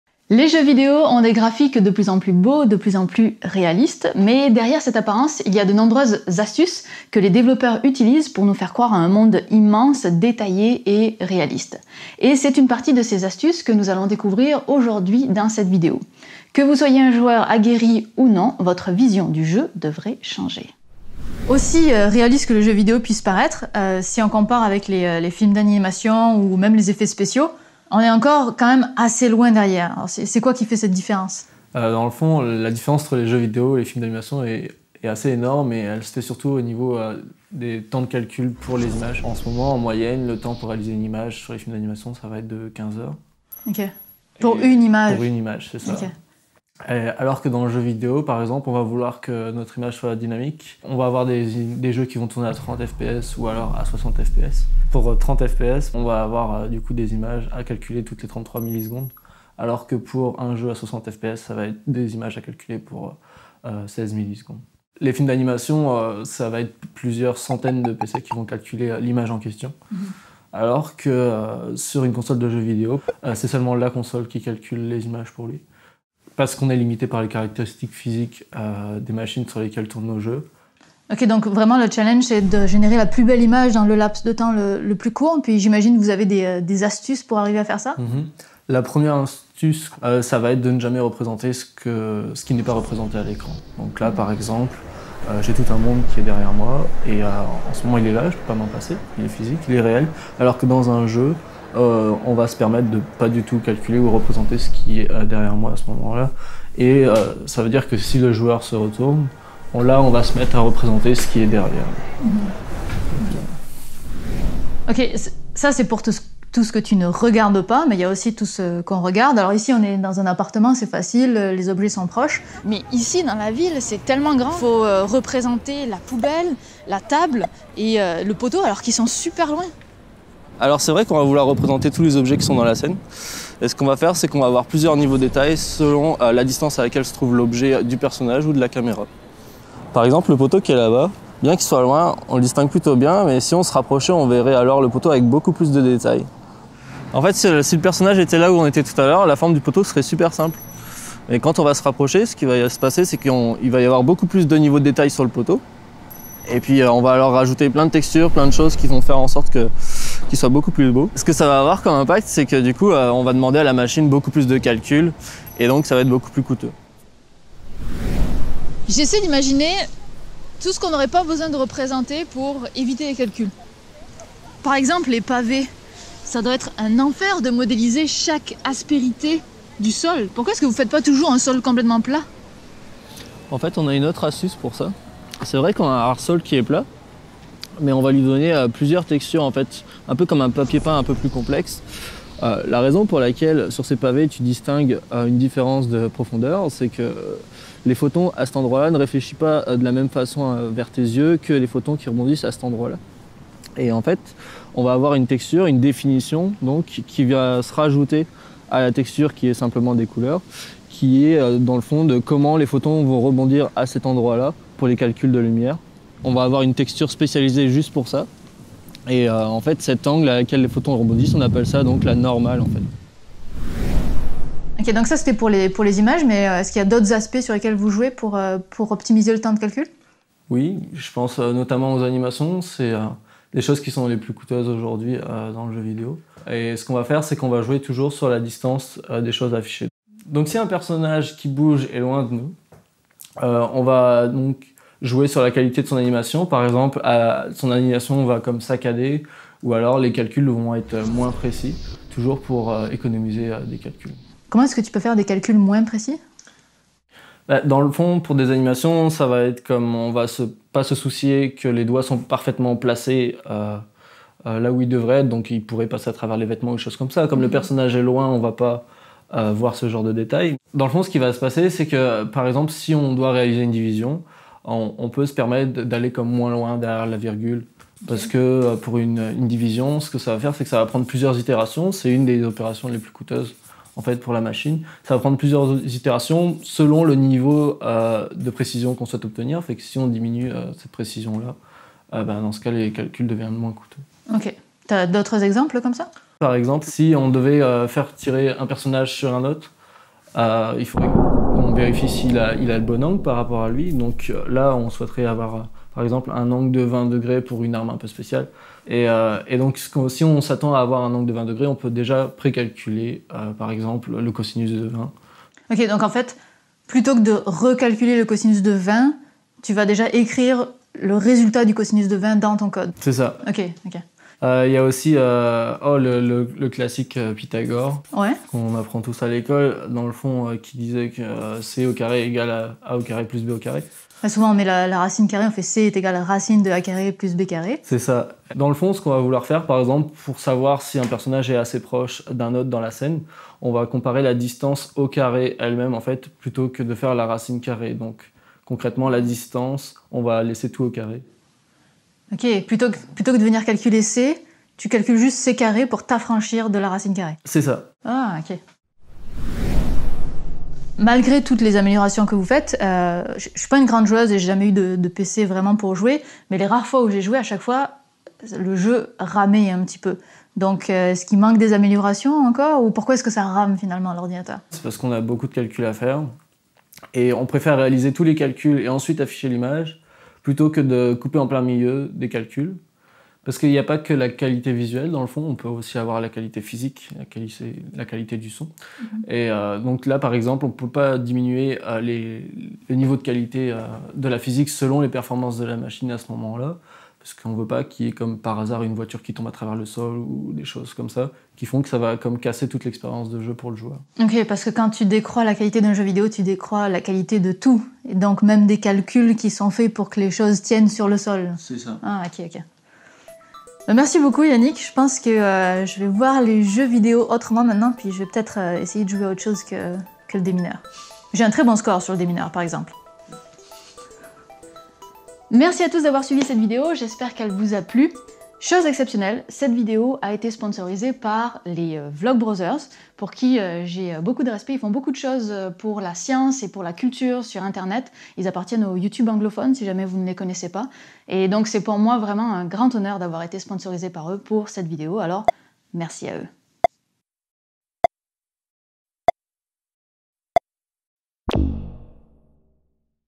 Comment les jeux vidéos parviennent-ils à être aussi réalistes ?
Les jeux ont des graphismes de plus en plus magnifiques. Mais derrière cette esthétique se cachent de très nombreuses astuces techniques pour permettre à nos consoles et ordinateurs d'afficher le jeu. Avec l'aide de Yannick Triqueneaux, développeur de jeux vidéos chez Ubisoft, découvrez une partie de leurs astuces.

Note : j'aurais du dire "graphisme" et non "graphique" au début de la vidéo. Mes excuses à tous ceux que cela a choqué !

Crédits
Mixage son : Aurélien Beliard
Animations : Yannick Triqueneaux et Viviane Lalande
Caméra : Manuel Vonthron
Remerciements : Manuel Vonthron et Léo Grasset
Un grand merci au Vlogbrothers pour leur soutien !

Scilabus est une chaîne qui parle de science en faisant des expériences. Ce que vous apprendrez vous fera briller en société !